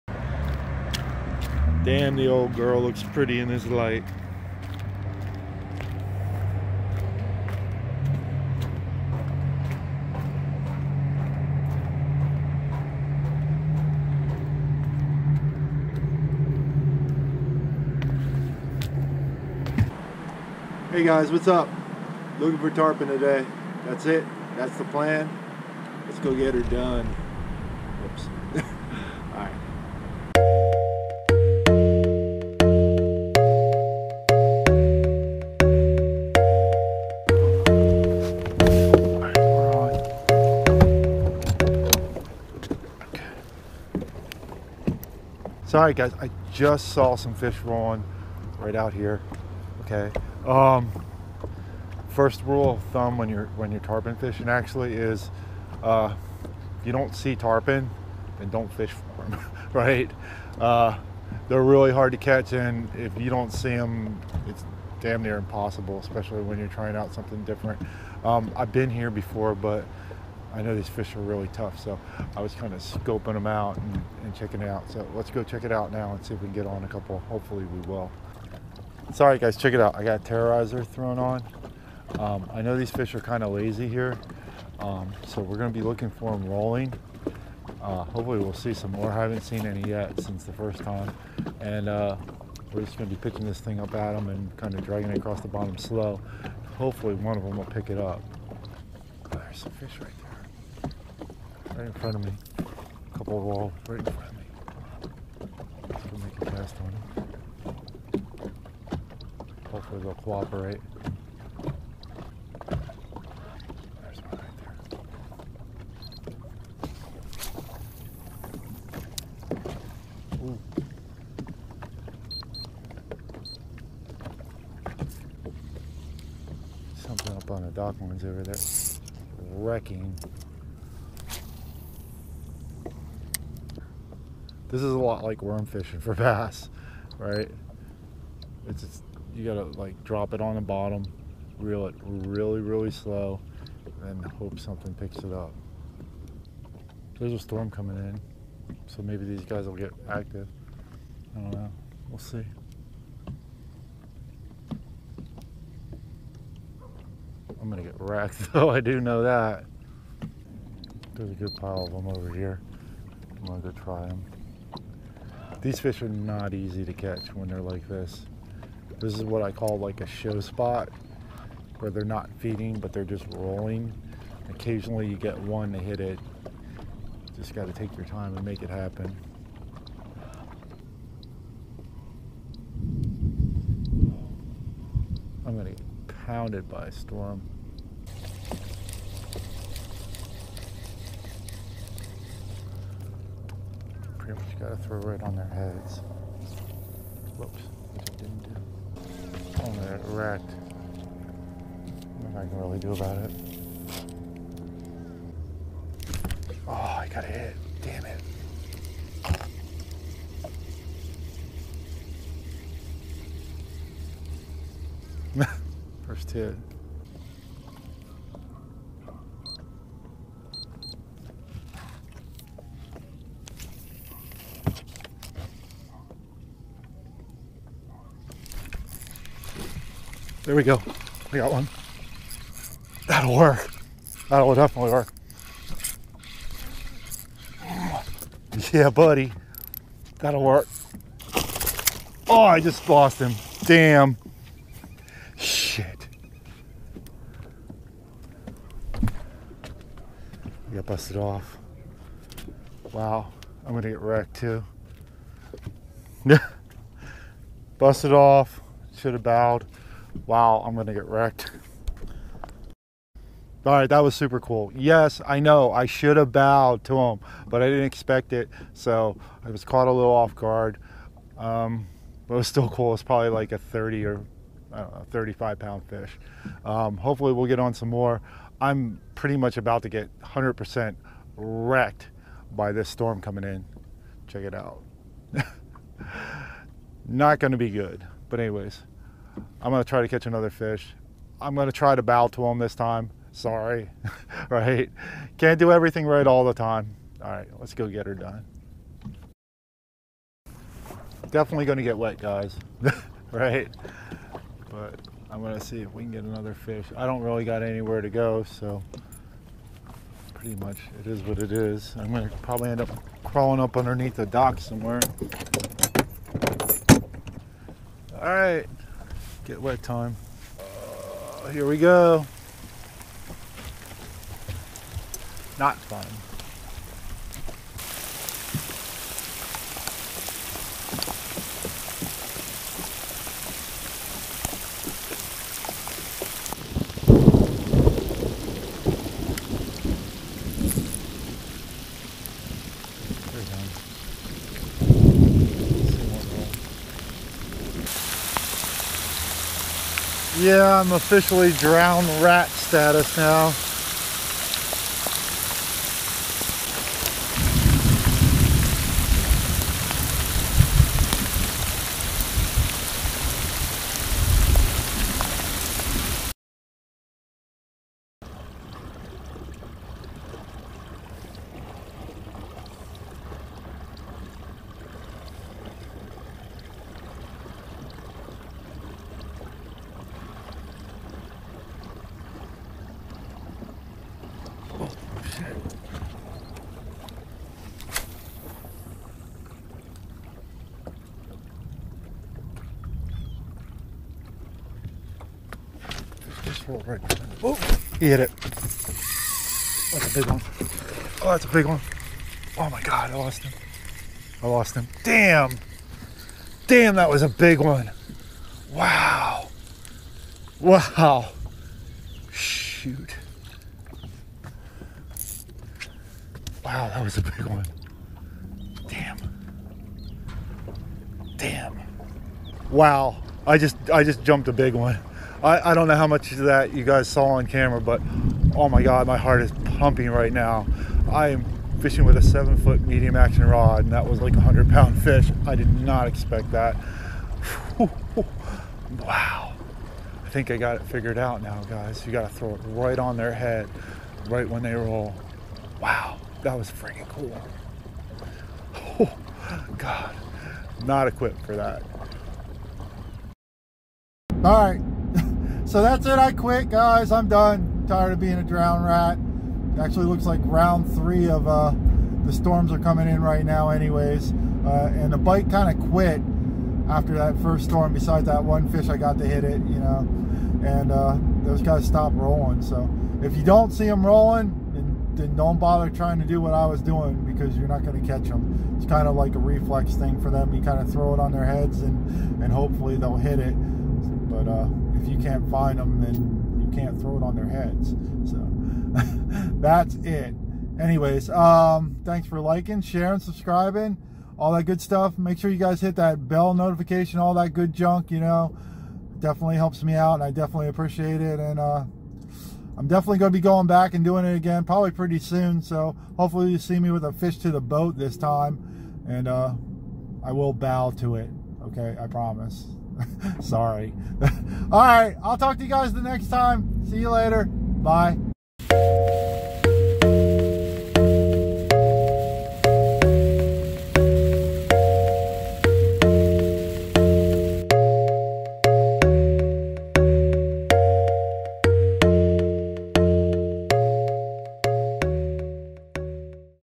Always BOW to The KING (Tarpon Fishing with Artificials)
I have been tarpon fishing a bunch lately. Although I didn't get a fish to the boat this video, I still think it's a good video to explain some techniques I use for catching these fish...

Hey guys, if you like the content do me a favor. It definitely helps me out and is greatly appreciated!!

Species Targeted.

Tarpon

Gear.

7' medium action St. Croix rod
Florida Fishing Products Osprey 3000 reel
10 lb. Powerpro braid
30 lb. fluro
DOA Terroreyz

Cameras.

Galaxy S9+